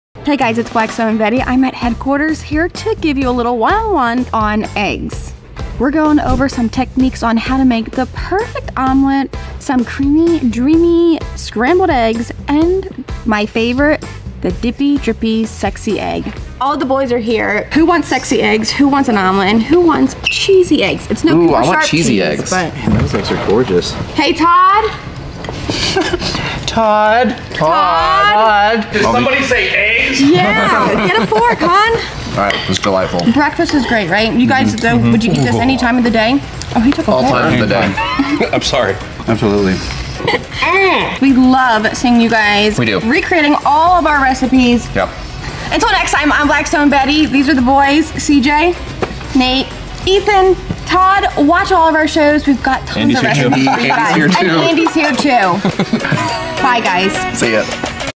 How to Make the Perfect Eggs on the Blackstone
Whether it be scrambled eggs, an omelette, or sunny side up; Blackstone Betty shows you how to make the perfect eggs on your Blackstone griddle - every single time!